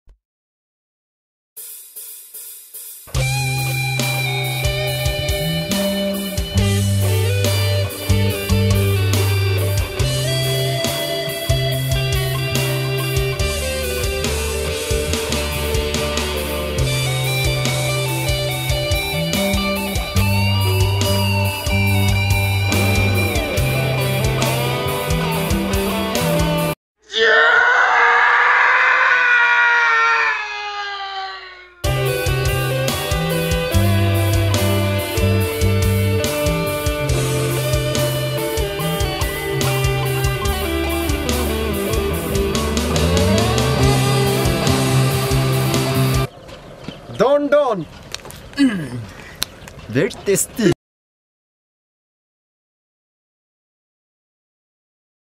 Funny commercial-DON DON (A MOREH Snack)
Shot,Edited and released in 2 hours.
Watch and share this funny ad of Don Don Snacks.
A very popular snack made from Myanmar Burma.